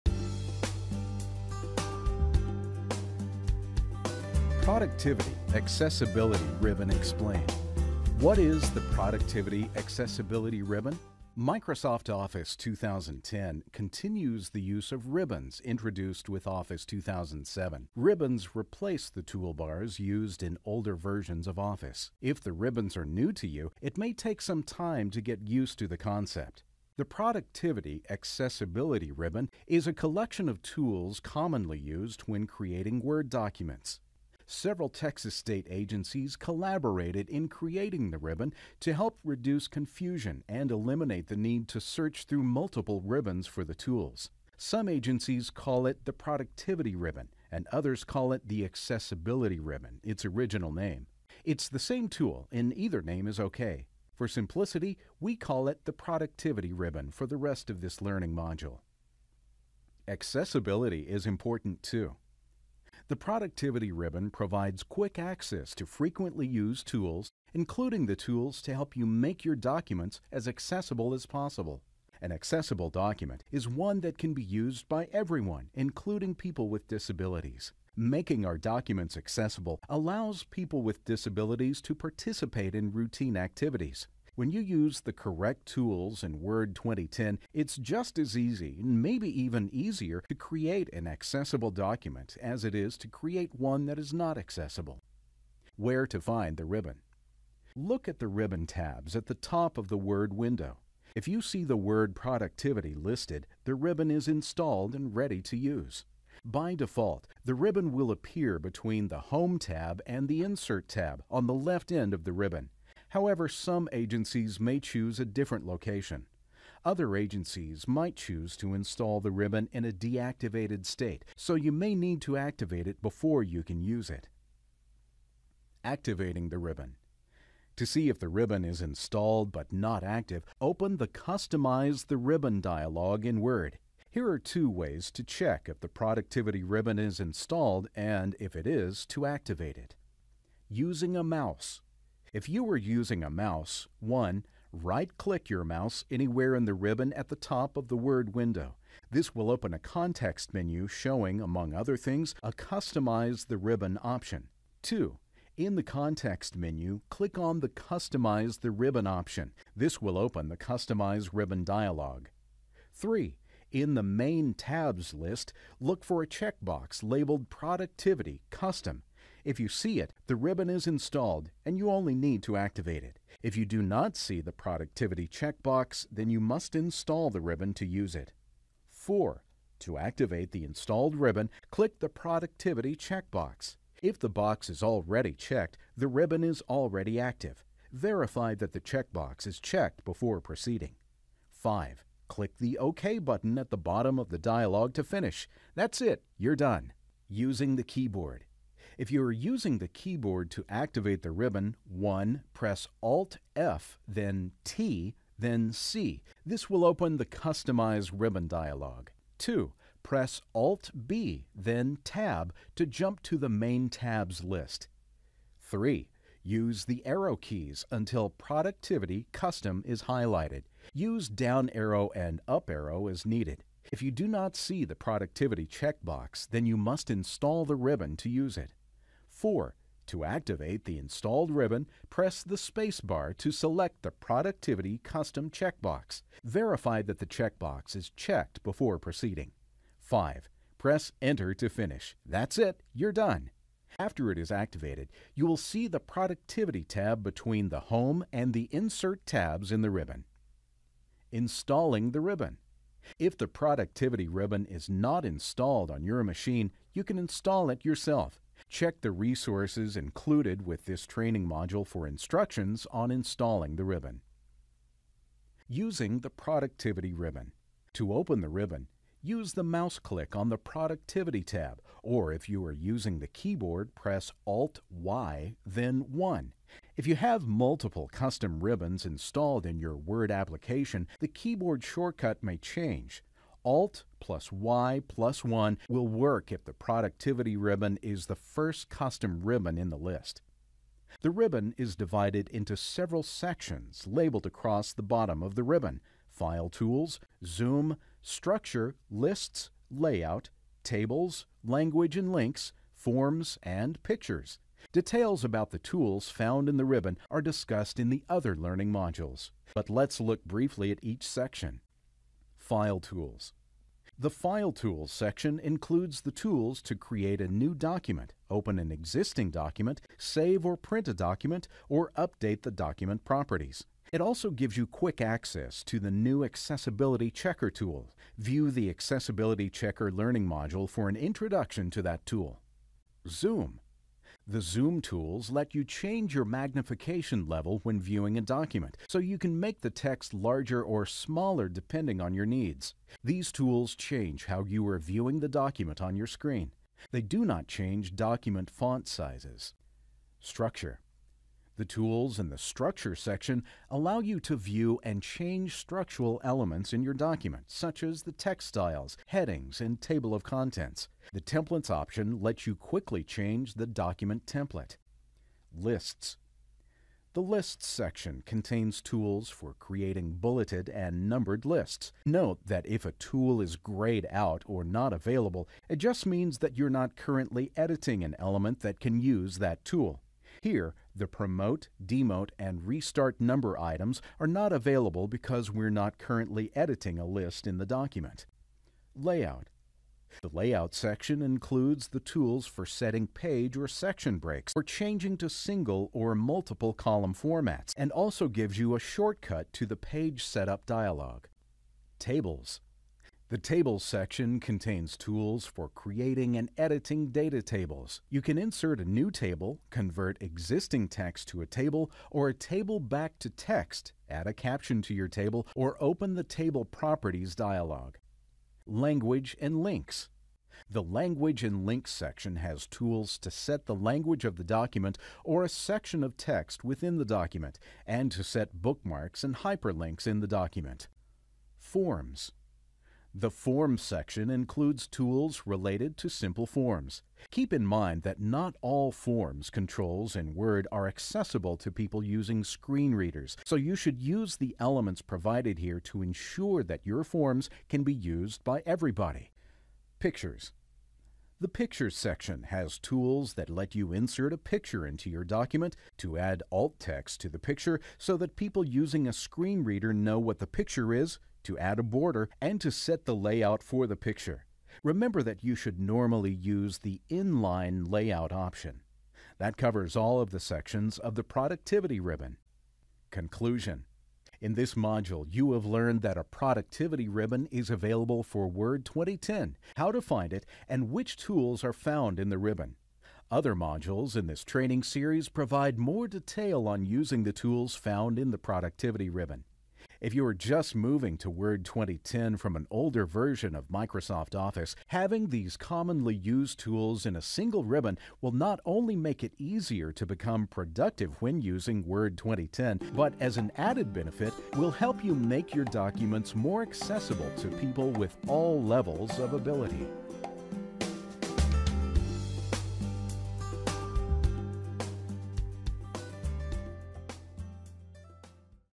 Creating Accessible Microsoft Word 2010 Documents: Accessibility/Productivity Ribbon Explained
This is one of a collection of learning modules created by the collaboration of a group of Texas state agencies. The Productivity or Accessibility Ribbon is a collection of commonly used tools to create an accessible document. This module explains how to activate the Ribbon, and summarizes the function of each Ribbon group.